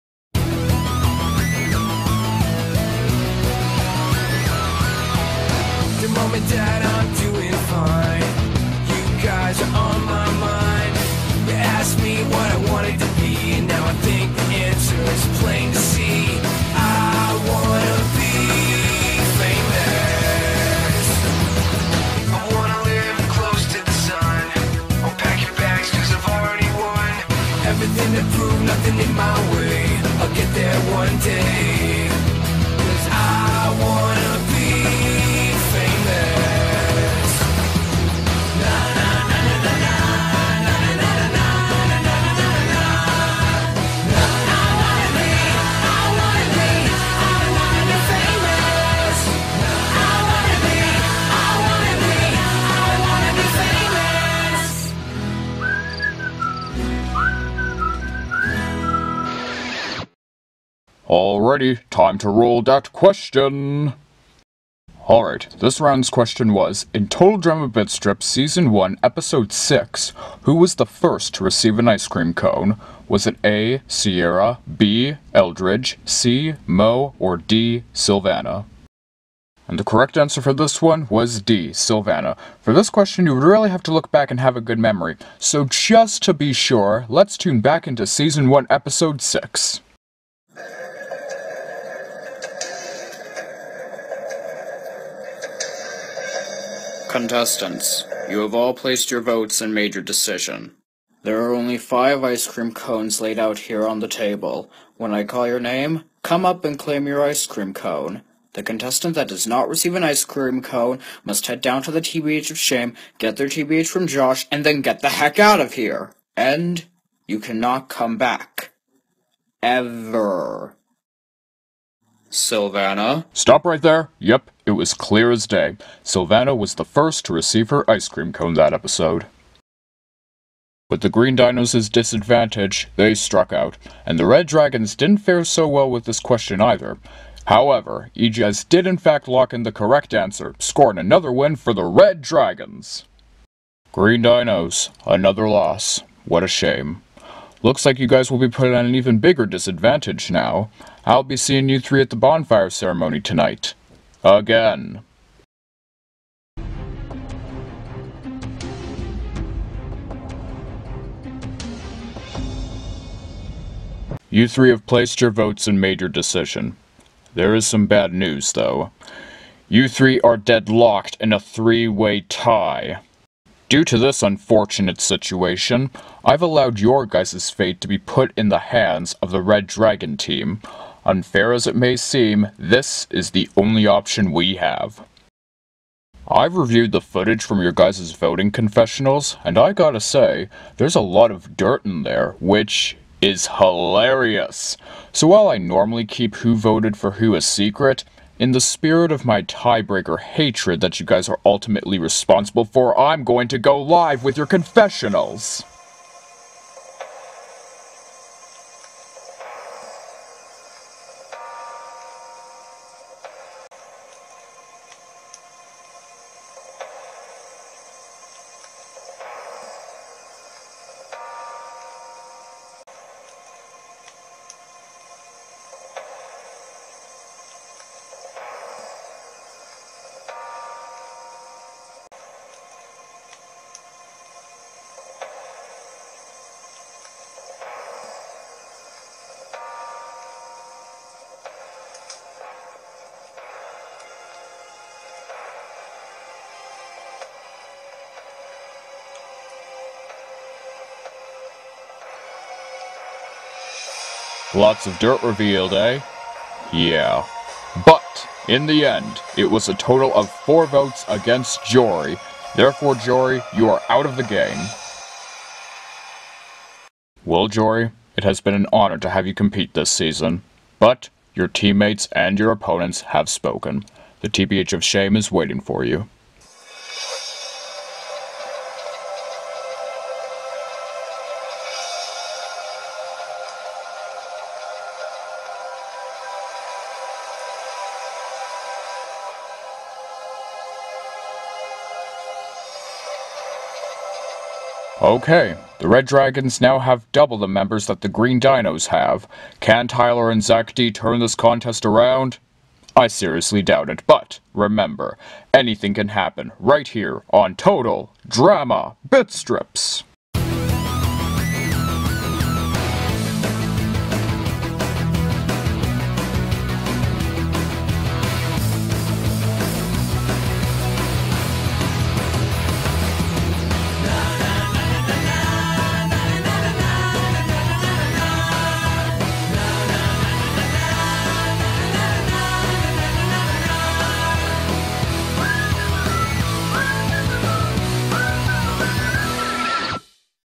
Total Drama Bitstrips Season 3 Episode 5
The Red Dragons have snagged victory once again after a challenging question. With only three members of the Green Dinos left, they find themselves in a deadlocked vote. So Bob decides to give the power to the Red Dragons. Who will they decide to cast off to the TBH Of Shame?